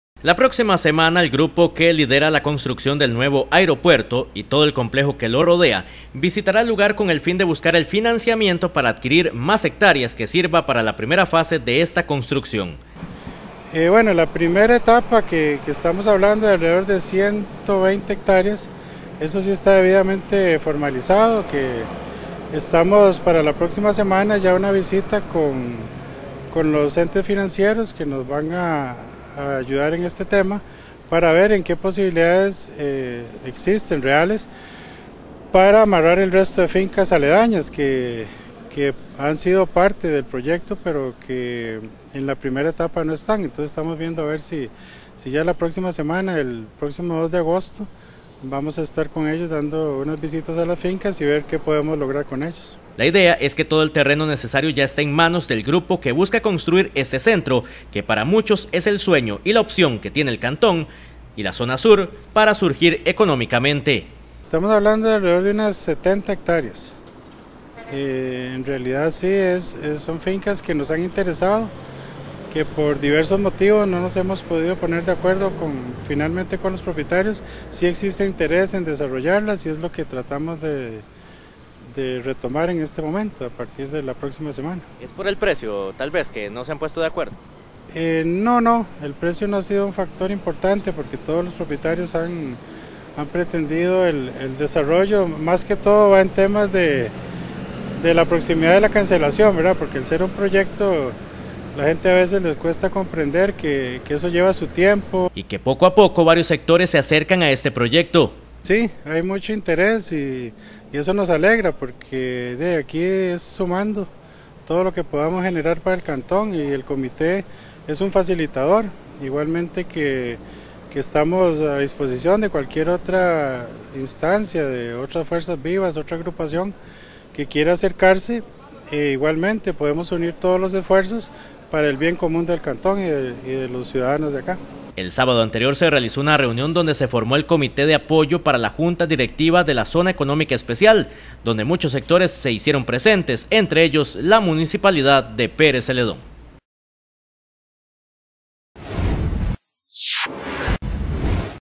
Nuevo aeropuerto ya tiene 120 hectáreas y busca 70 más
La próxima semana el grupo que lidera la construcción del nuevo aeropuerto y todo el complejo que lo rodean,visitará el lugar con el fin de buscar el financiamiento para adquirir más hectáreas que sirvan para la primera fase de esta construcción.